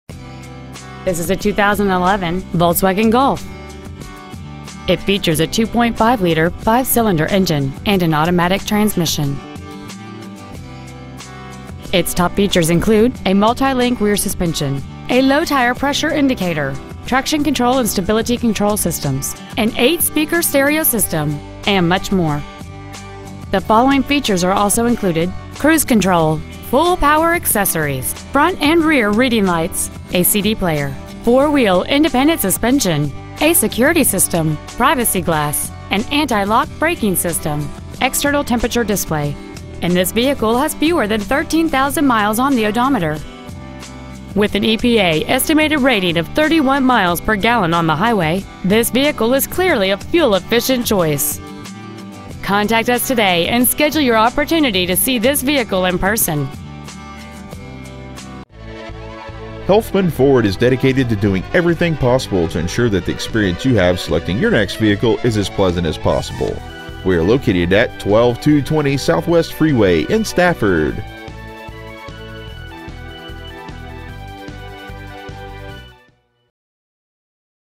2011 Volkswagen Golf Stafford TX
We are proud to present this 2011 Volkswagen Golf .

 Year : 2011
 Make :
 Model :
 Engine : 2.5L I5 20V MPFI DOHC
 Trans . : 6-SPEED AUTOMATIC
 Exterior : REFLEX SILVER METALLIC
 Miles : 12,755
 Interior : GRAY
 Stock : PD091A